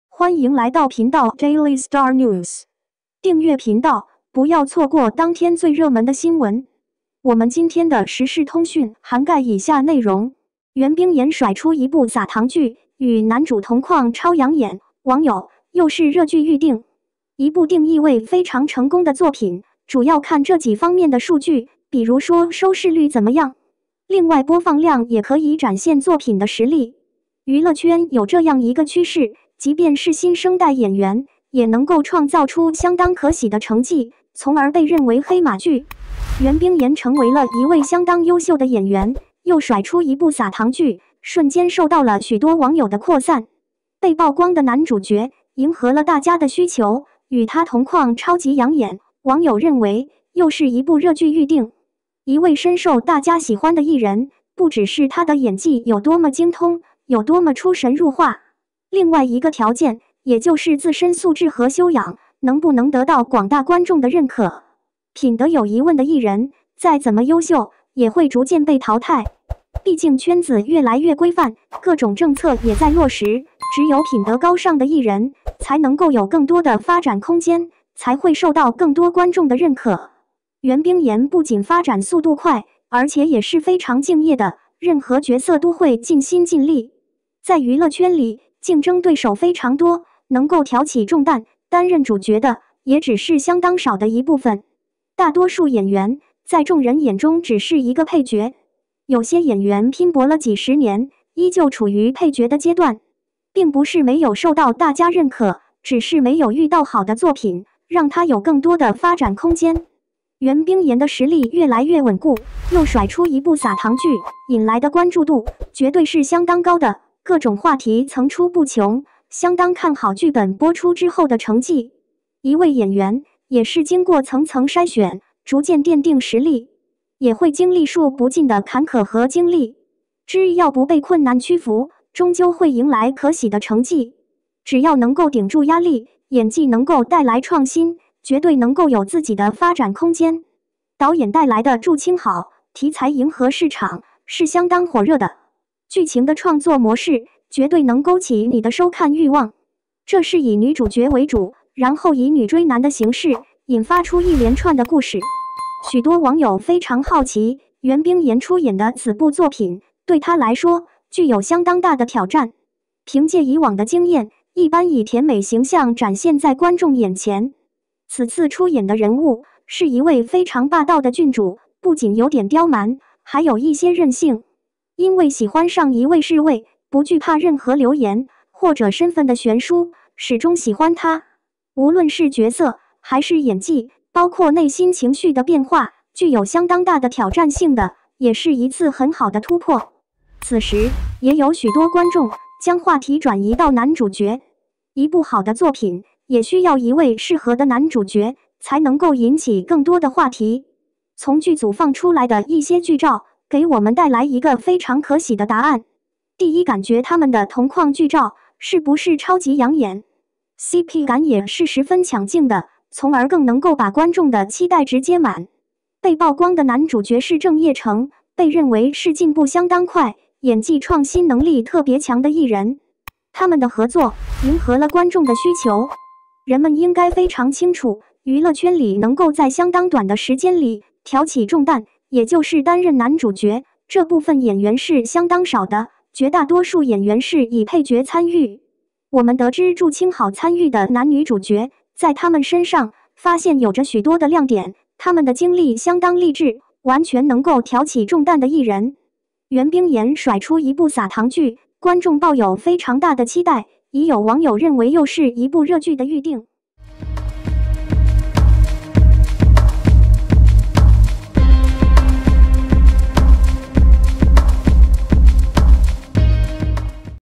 袁冰妍甩出一部撒糖剧，与男主同框超养眼，网友：又是热剧预定
袁冰妍甩出一部撒糖剧，与男主同框超养眼，网友：又是热剧预定  | Daily Star News